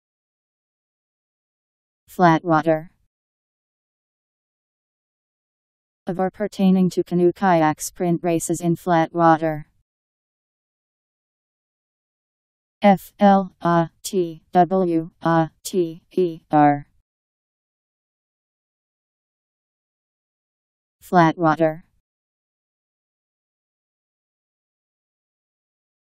Flatwater Meaning
Video shows what flatwater means. Of or pertaining to canoe-kayak sprint races in flat water..  Flatwater Meaning. How to pronounce, definition audio dictionary. How to say flatwater. Powered by MaryTTS, Wiktionary